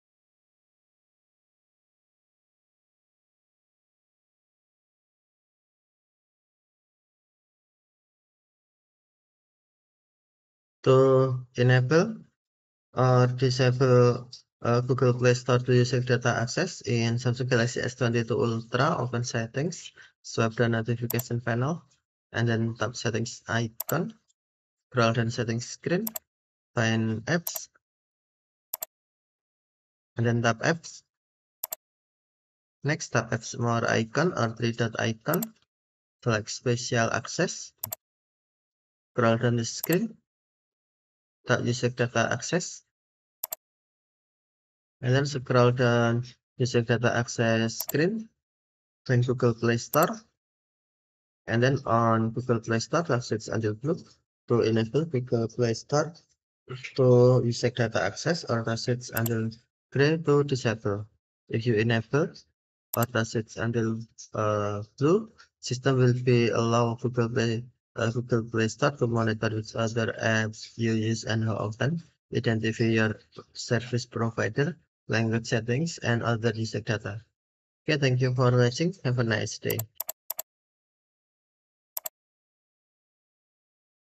Samsung Galaxy S22 Ultra : How to Enable or disable Google Play Store to Usage data Access
In this video, you will find the steps on How to Enable or disable Google Play Store to Usage data Access in Samsung Galaxy S22/S22+/S22 Ultra.  This video uses Samsung Galaxy S22 Ultra SM-S908E/DS International version runs with Android 12